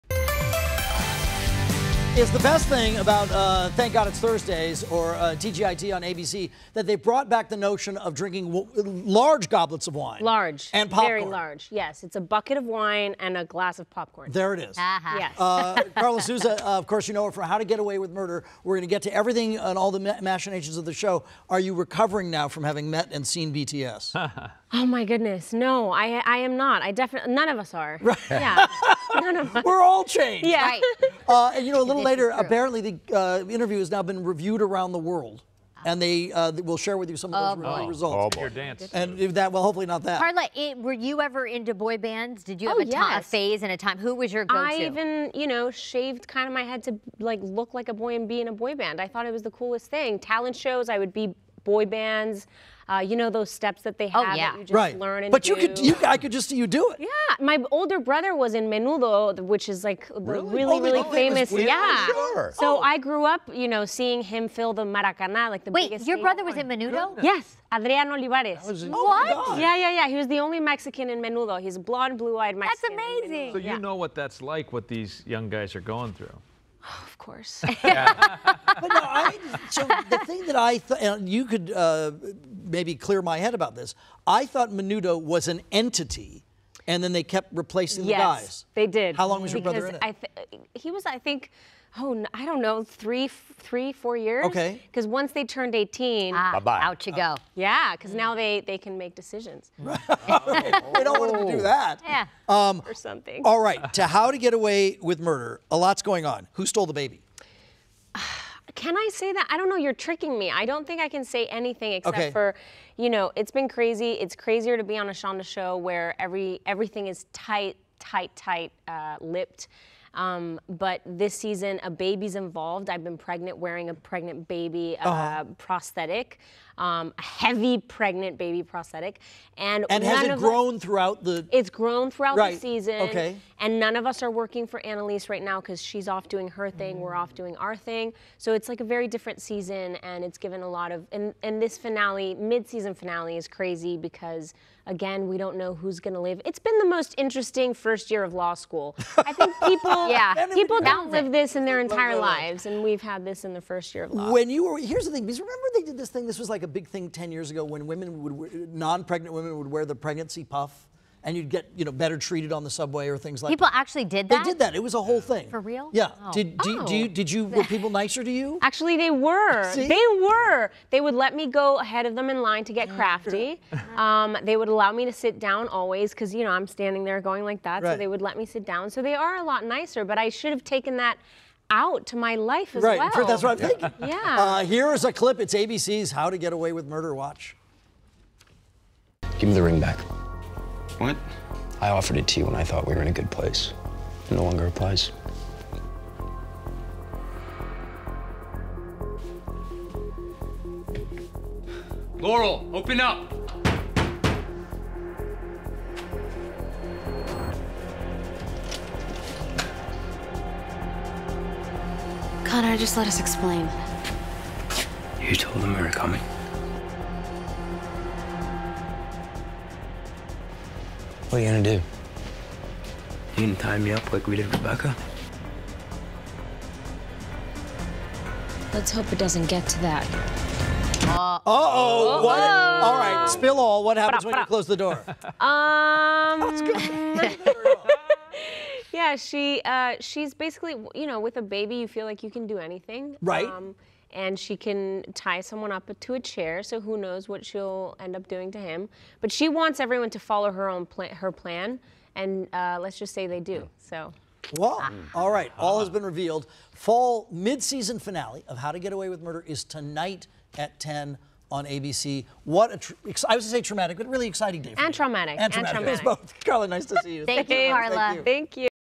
Karla Souza Spills on the Special Treatment from Fake Pregnancy on HTGAWM
Don't miss the fall midseason finale of "How to Get Away with Murder" tonight at 10 on ABC. This segment aired on the KTLA 5 Morning News, Thursday, November 16, 2017.